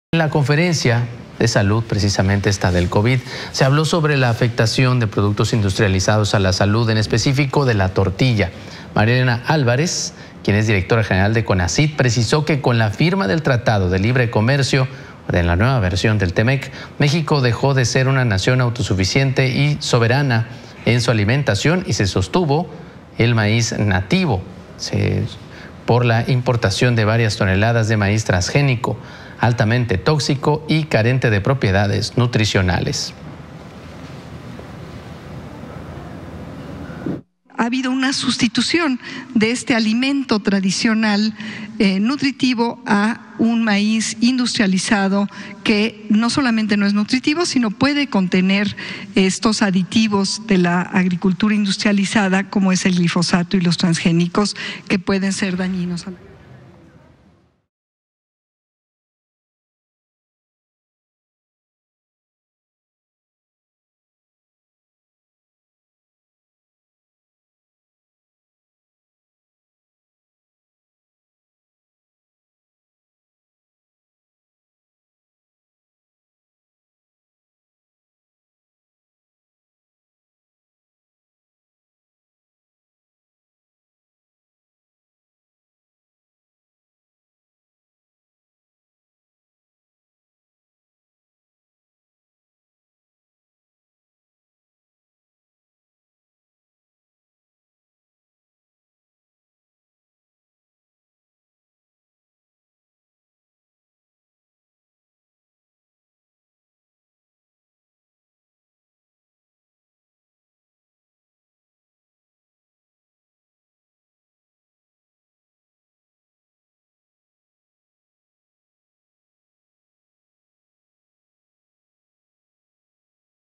Tortillas industrializadas son tóxicas y provocan daños a la salud: Conacyt
La titular del Conacyt, María Elena Álvarez-Buylla, aseguró que el maíz nativo tradicional contienen elementos más saludables para la dieta mexicana; dijo que el maíz industrializado contiene exceso de almidones, que provoca la obesidad.